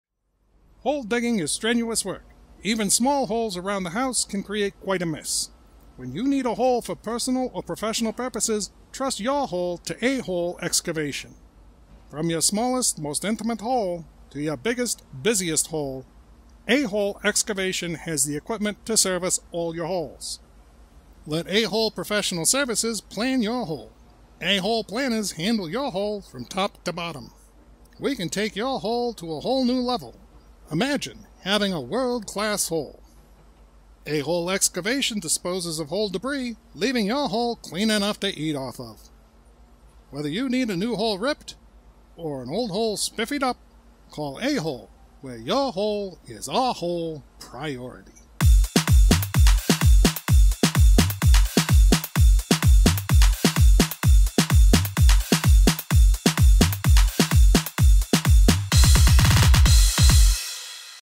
AHOLE EXCAVATING
Trust AHOLE for all you hole requirements. AHOLE, first in the phone book (in locations that still make phone books).  AHOLE will take your hole to a whole new level!

Created by David Hayden